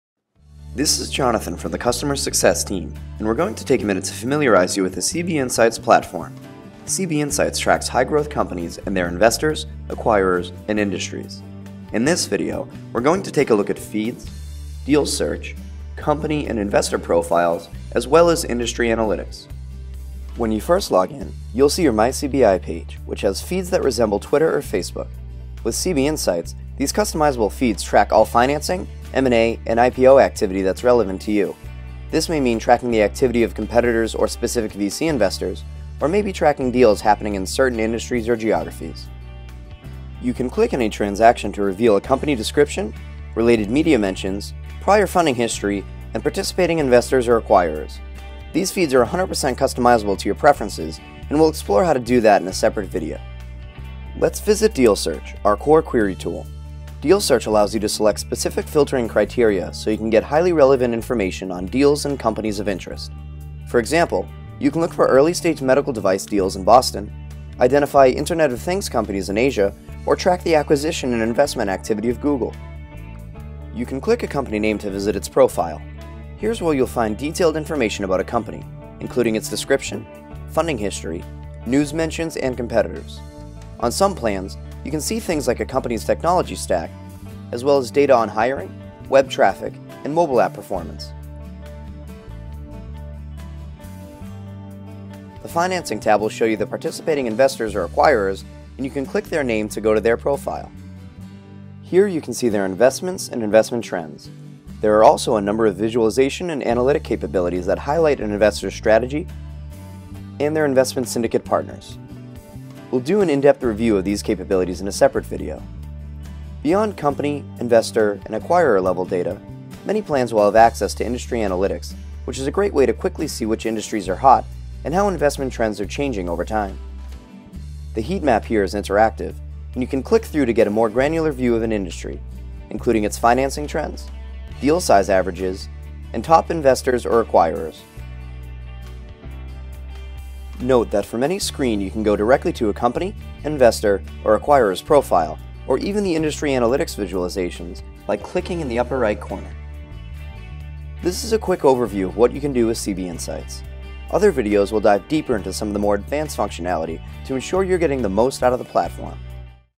Getting Started with CB Insights
CB Insights tracks high growth companies and their investors, acquirers and industries.  In this video, we're going to familiarize you with that platform, as we’re going to look at Feeds, Deal Search, company/investor profiles and Industry analytics.